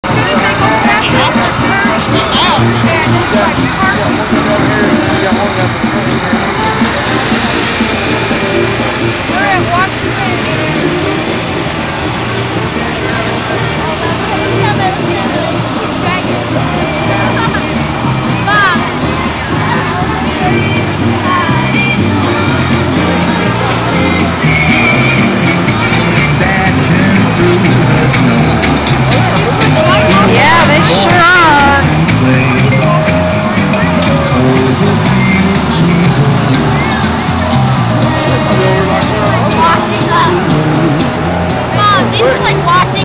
FESTIVAL OF LIGHTS
short clips of the lights parade